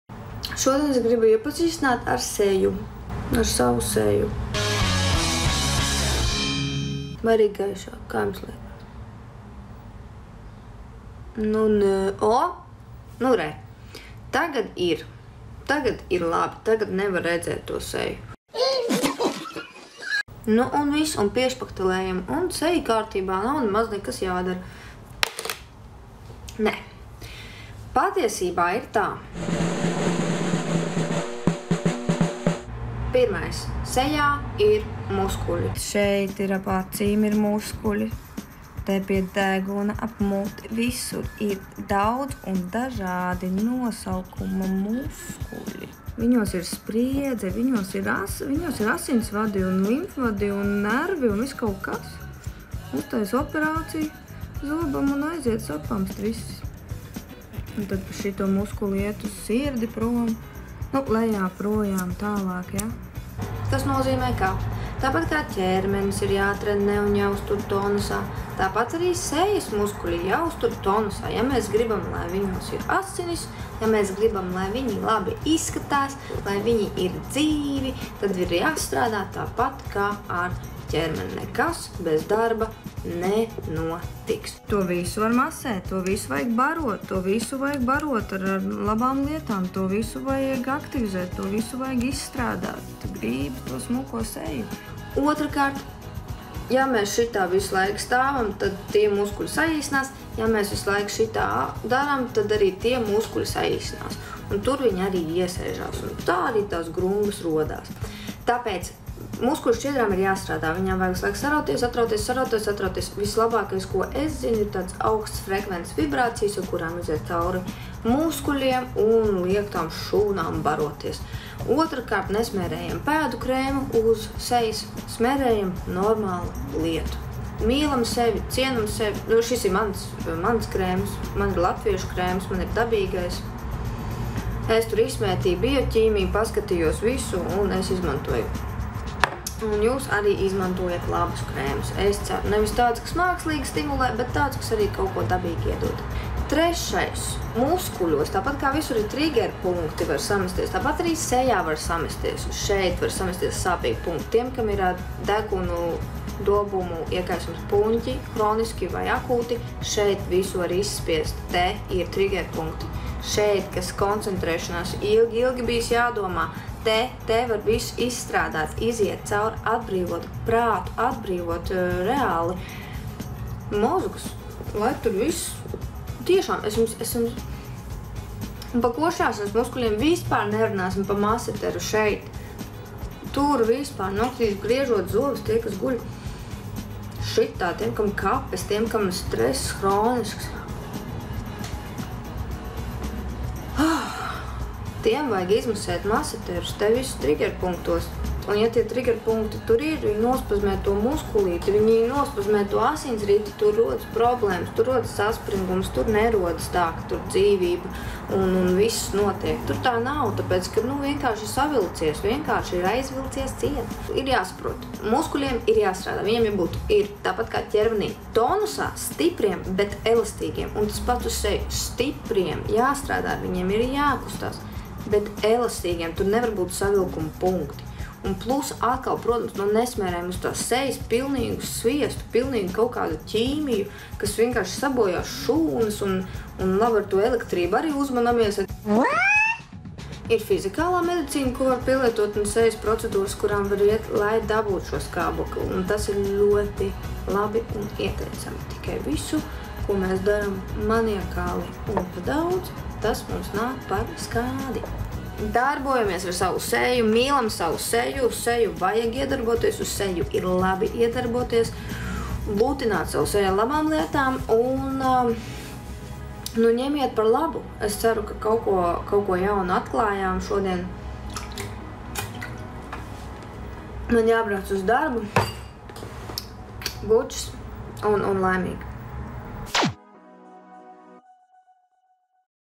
Kusties ar Agnetu. Ep11 – Seja
Ja trenējam kāju vai roku muskuļus, kāpēc kāds domā, ka sejas muskuļi nav jāuztur tonusā? “Hattrick ar Puči” ķermeņa eksperte Agneta Liepājniece saka, ka šādām domām jāpieliek punkts!
Konkrēti padomi, ko darīt lietas labā noderēs ikvienam. Jā arī sīvajiem večiem 😉
Nekas bez darba nenotiks!